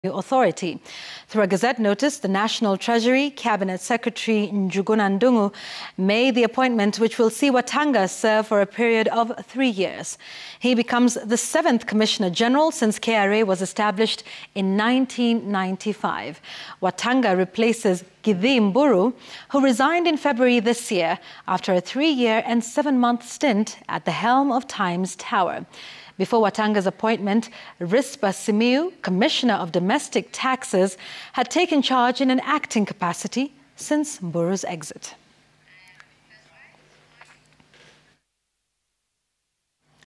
Humphrey Wattanga appointed as the new Commissioner General for KRA
Through a gazette notice, the National Treasury Cabinet Secretary Njuguna Ndung'u made the appointment which will see Wattanga serve for a period of three years. He becomes the seventh Commissioner General since KRA was established in 1995.  Wattanga replaces Githii Mburu who resigned in February this year after a 3 year and seven-month stint at the helm of Times Tower. Before Wattanga's appointment, Rispah Simiyu, commissioner of domestic taxes, had taken charge, albeit in an acting capacity since Mburu's exit.